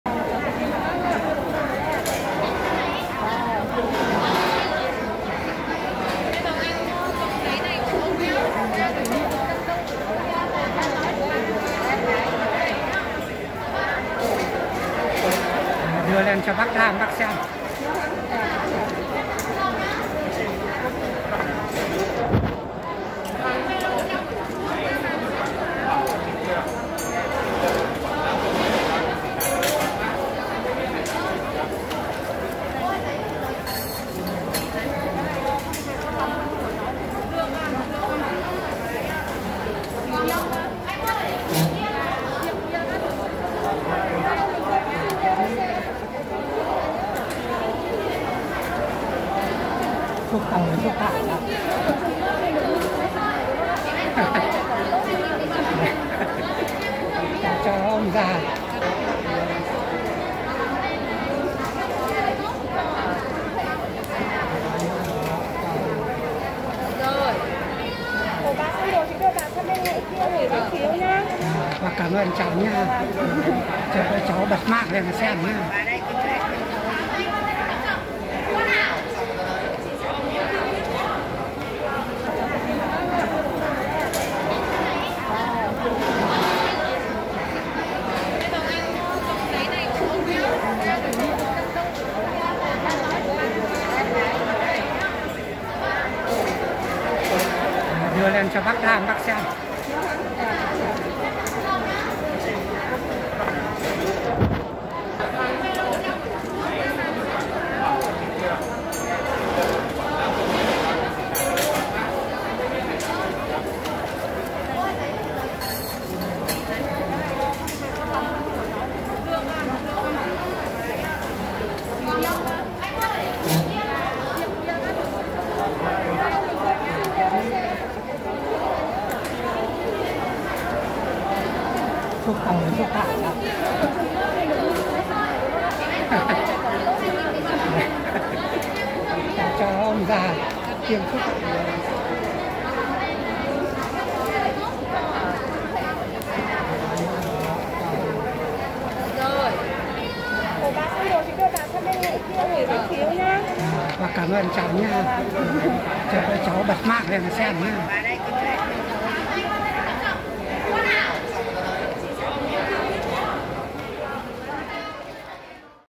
Hà Nội phòng chống dịch covid-19
Tiêm vắc xin thần tốc ngày 11-9-21 tại Hà Nội Quyết tâm đẩy lùi giặc covid Ra khỏi lãnh thổ Việt Nam